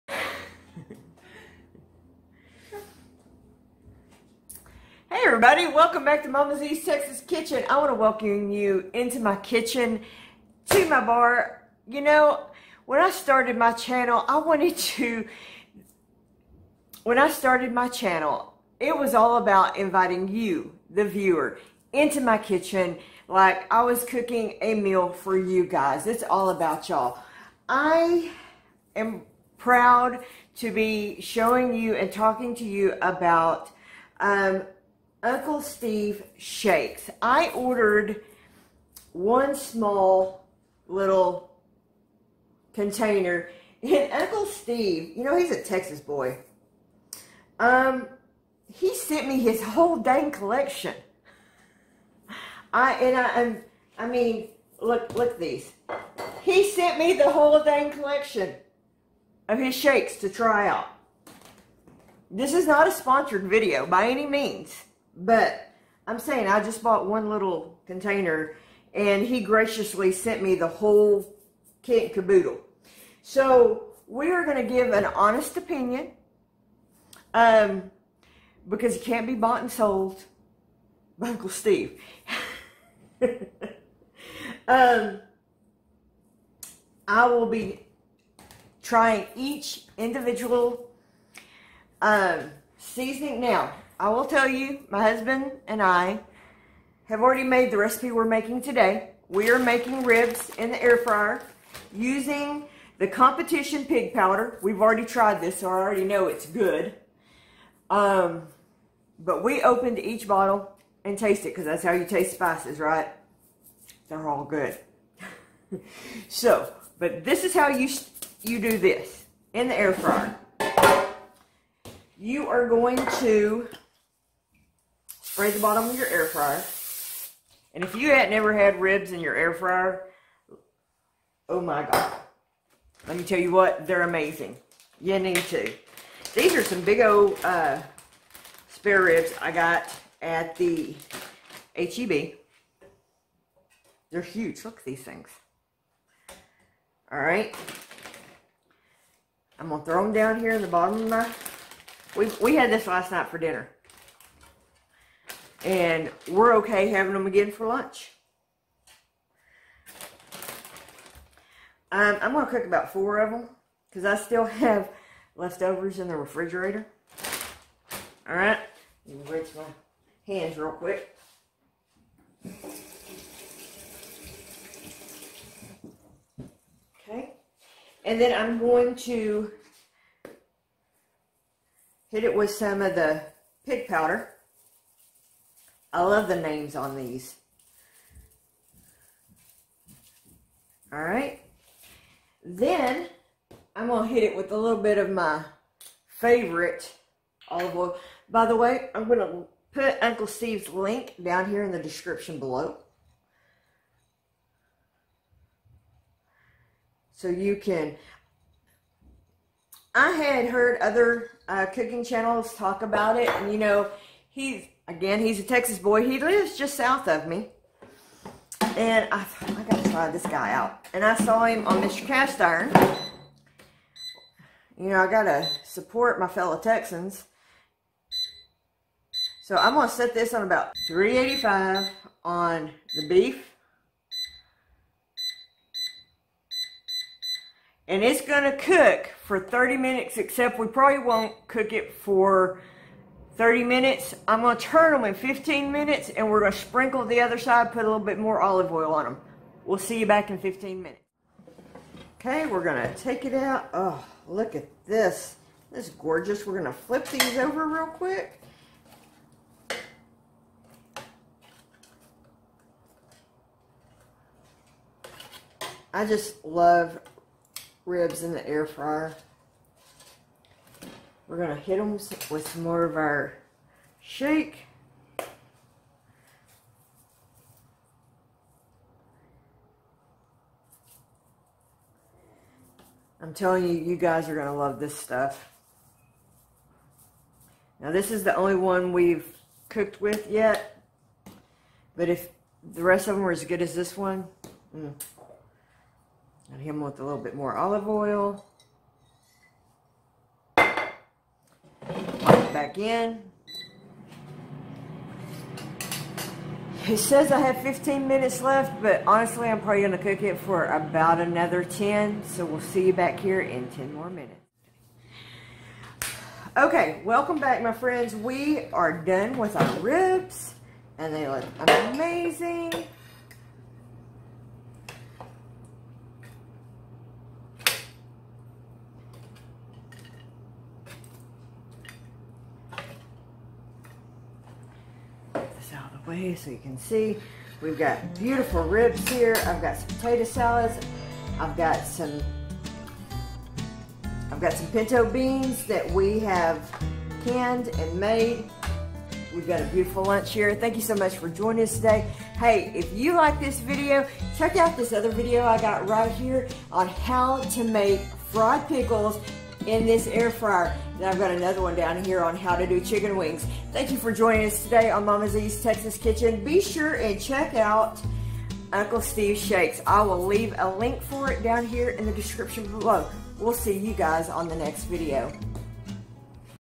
Uncle Steve Pig Powder Ribs In my Air Fryer!
We try Uncle Steve Pig Powder on Ribs In my Air Fryer! I'm leaving a link to Uncle Steves's web page below, We are loving what we have tried so far!

📢If you would like to help buy supplies for my channel you can do that here! 100% goes back into my channel!